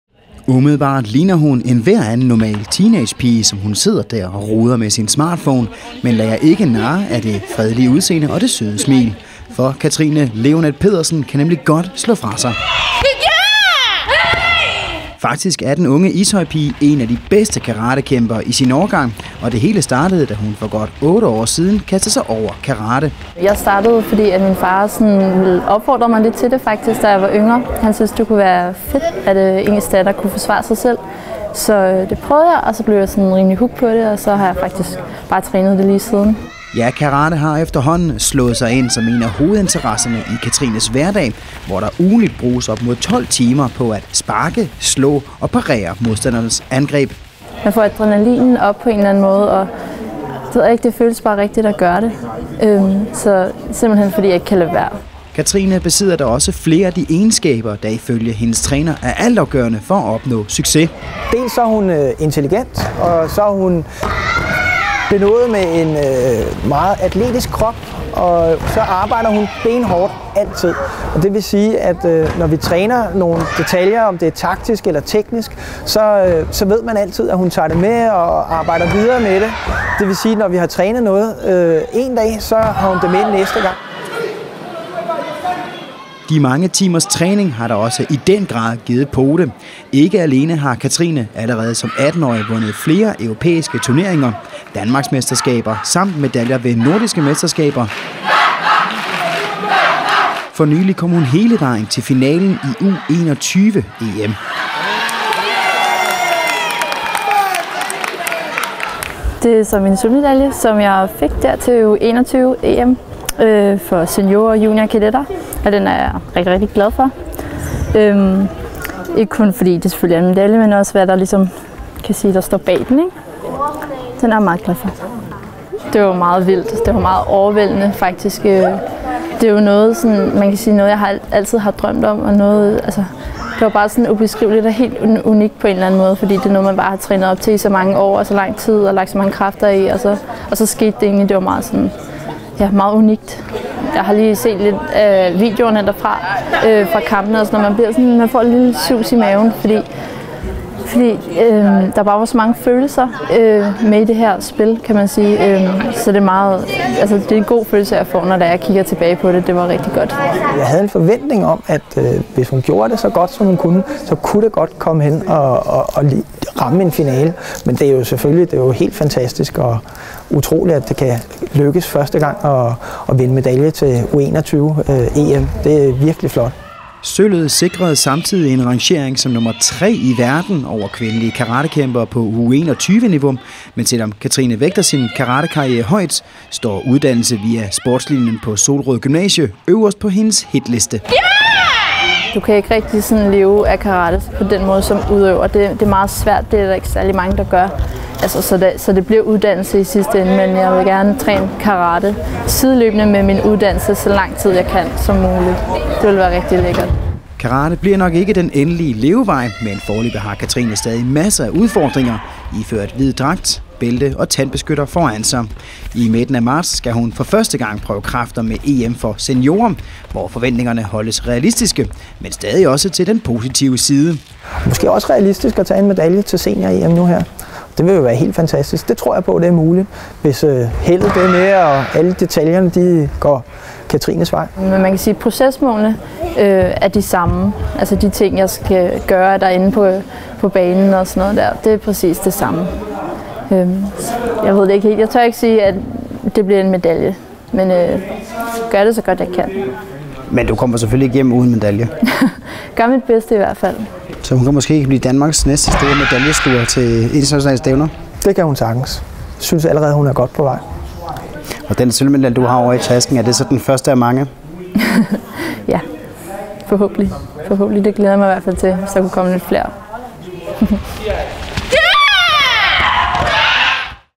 Karate-Katrine blandt verdenseliten -TV-Ishøj
Her kan I møde en lokal teenager med slag i. 18-årige Katrine Lionett Pedersen er nemlig ikke helt som andre gymnasiepiger på hendes alder. Ishøj-pigen går nemlig til karate, og faktisk er hun så god, at hun er blandt verdens bedste og netop har vundet sølv ved U21-EM.